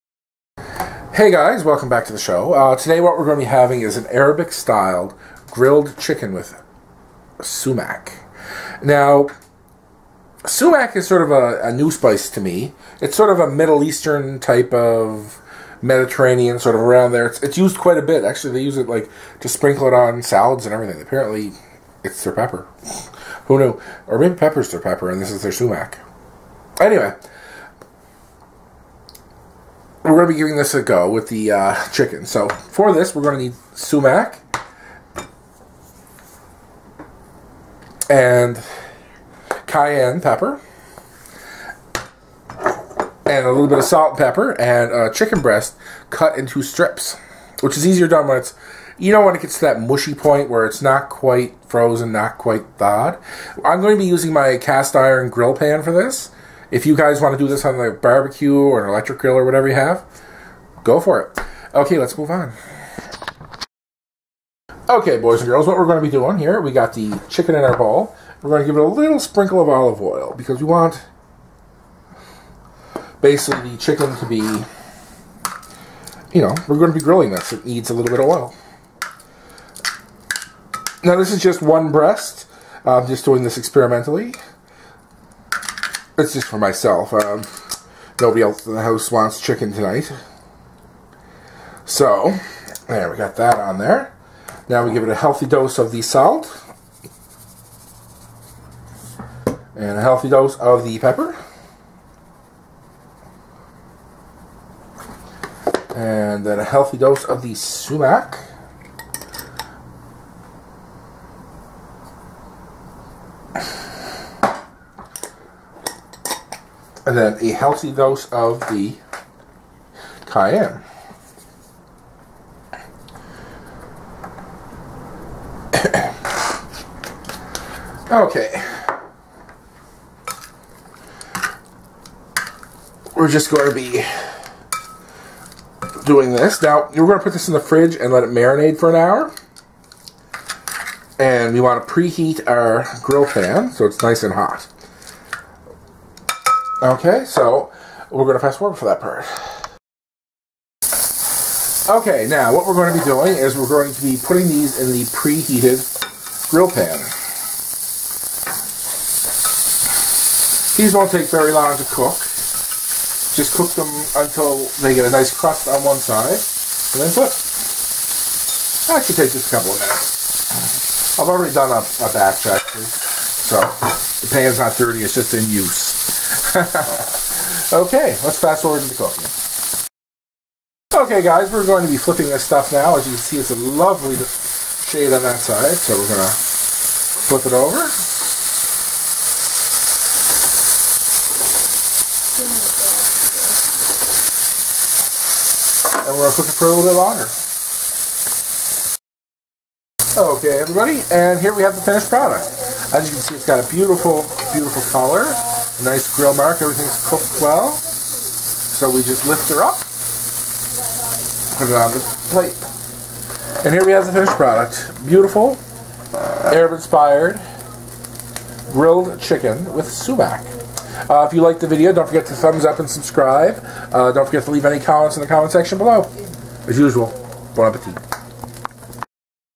sumac seasoned grilled chicken
Again, i went exptic and tried a new spice out, we have the delicious sumac spice, and decided to grill some chicken using it,  it came out delicous